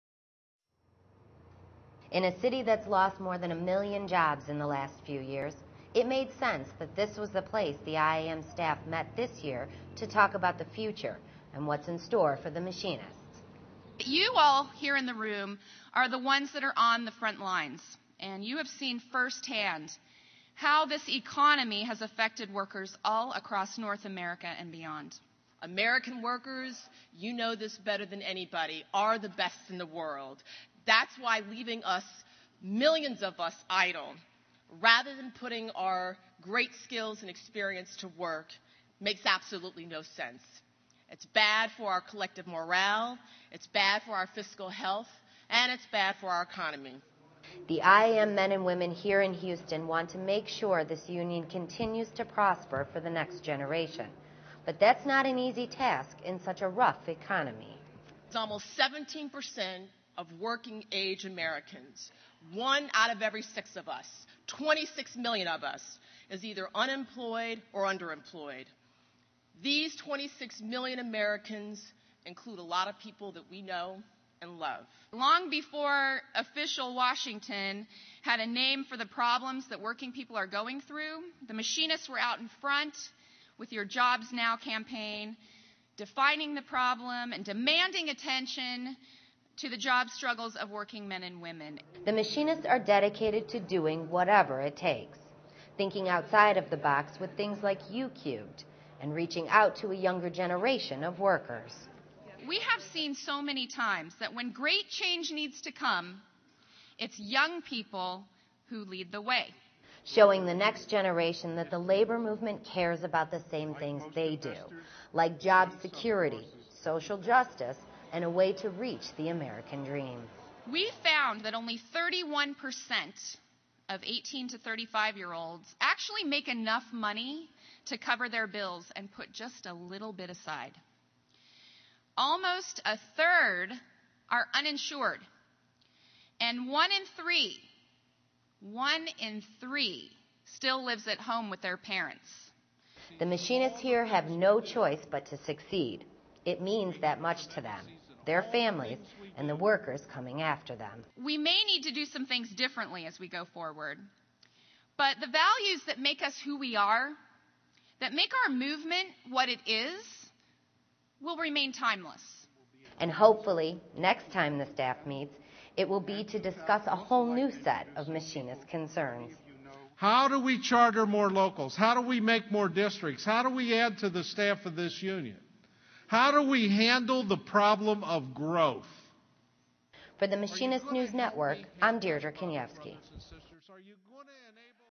Whatever It Takes
In a city that's lost more than one million jobs in the last few years, it made sense that this was the place the IAM staff met this year to talk about the future and what's in store for the IAM.